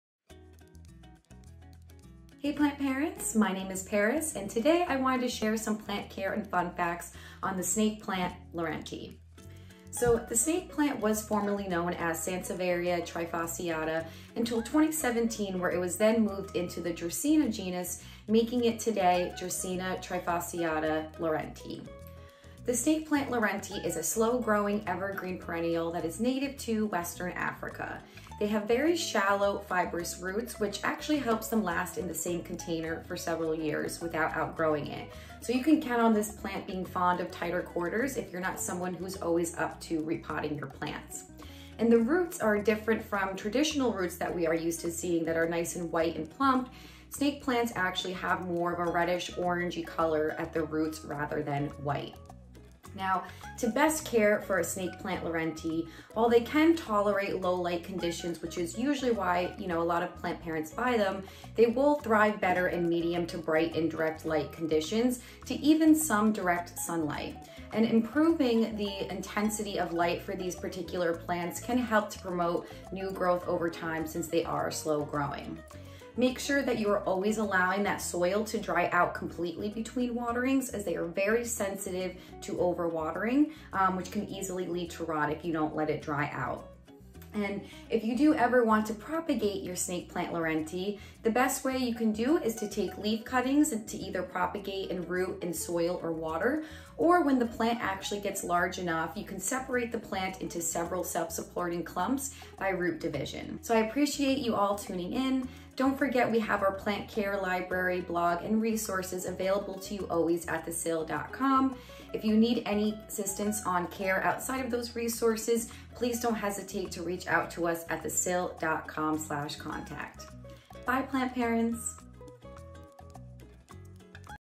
How to Care for a Snake Plant
Hello Plant Subscribers 👋 This Month's plant is the Snake Plant Laurentii. Learn more about this stunning plant with our in house plant expert, Paris!